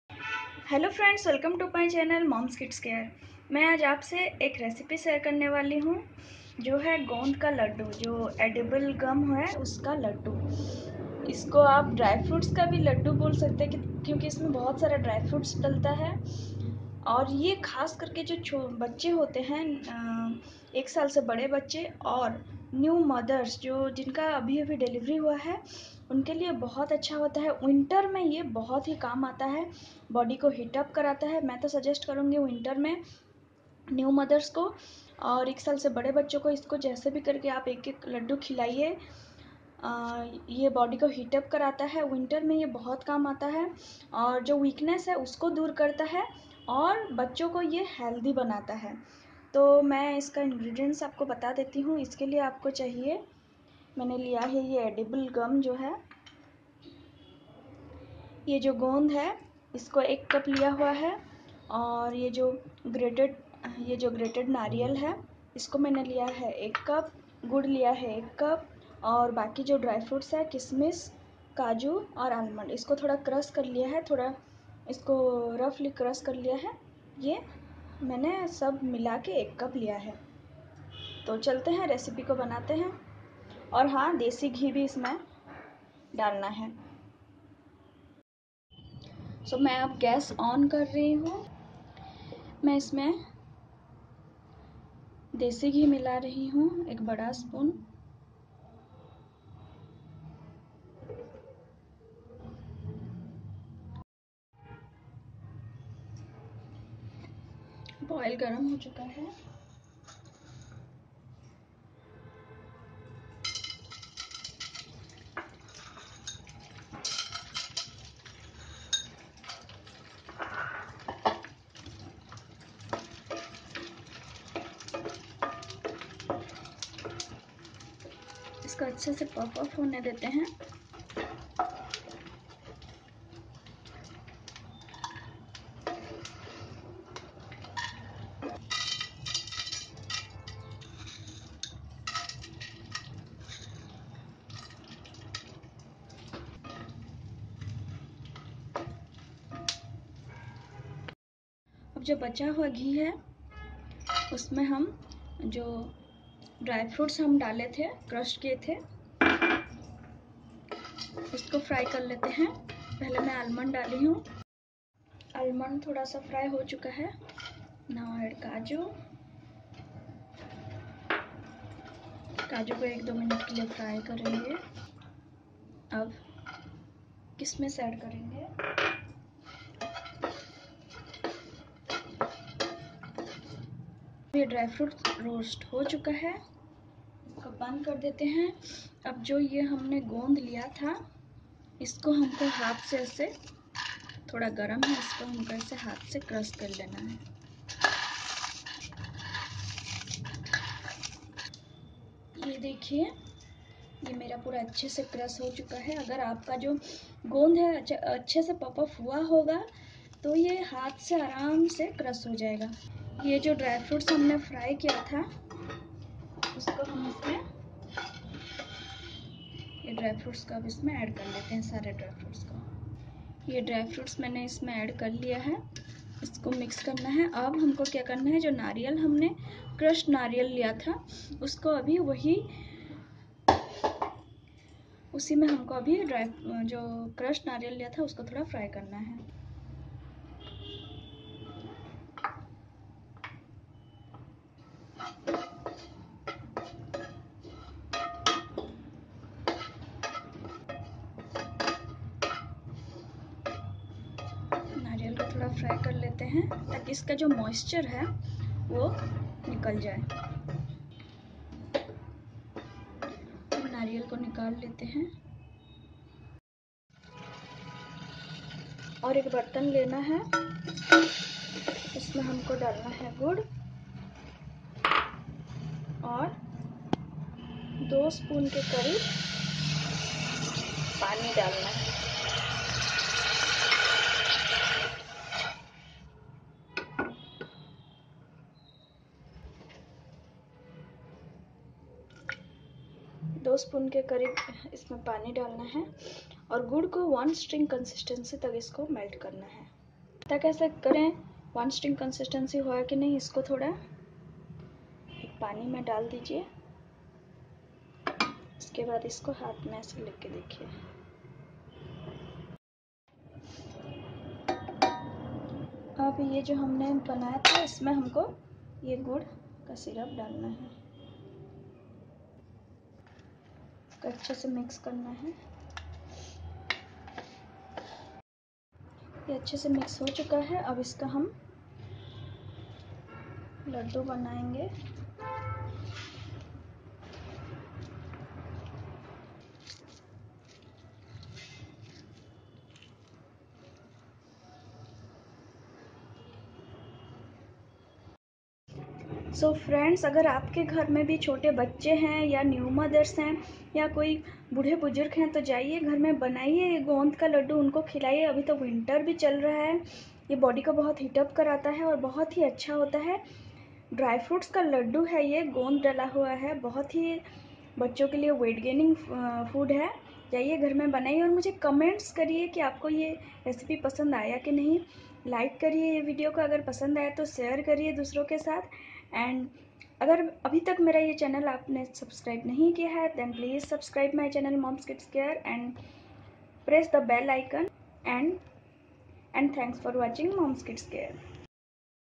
Baby wt gaining gaund ke ladoo recipe||Gond ke laddu after delivery||dry fruits ladoo recipe
gaund ke ladoo recipe:
Give this dry fruits laddoo to kids and women after delivery.It is an energy booster food.I hope you like this video.